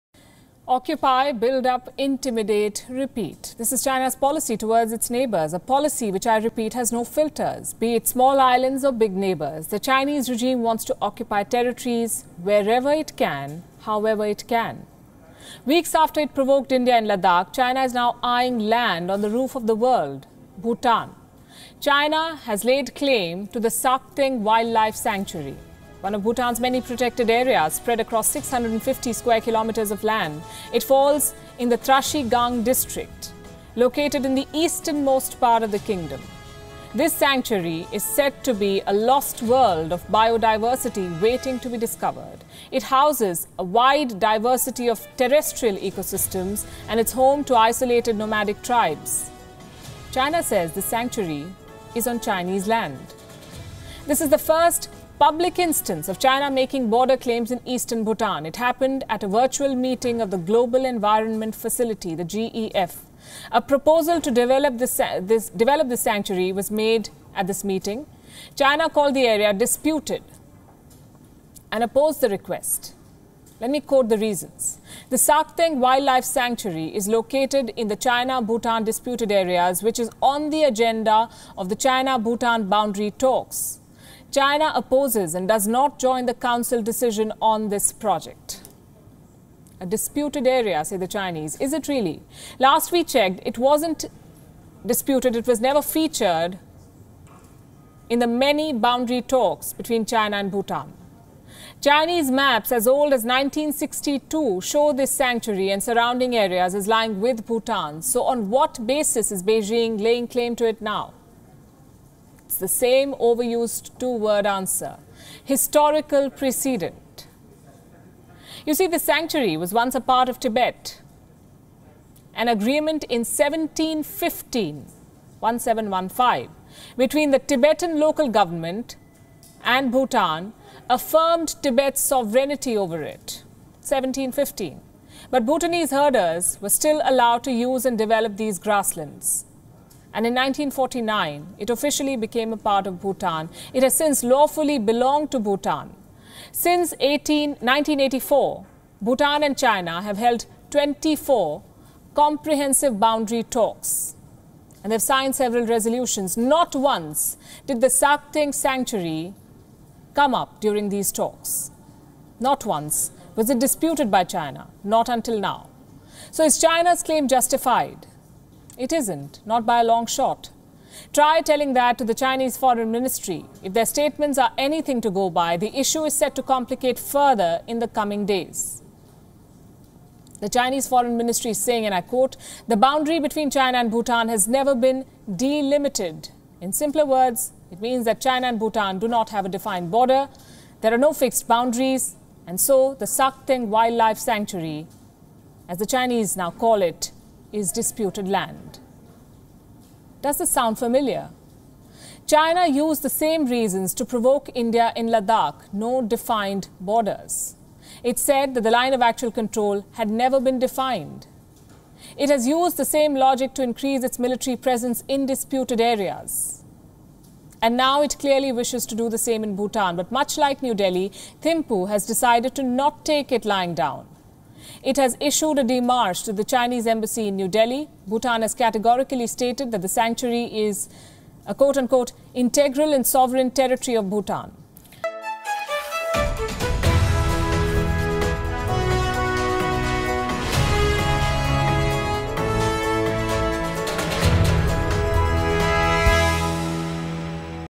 China claims Bhutan's territory
After India & Nepal, China has now laid claim to territories in Bhutan. Beijing says the Sakteng wildlife sanctuary in Bhutan is on Chinese land.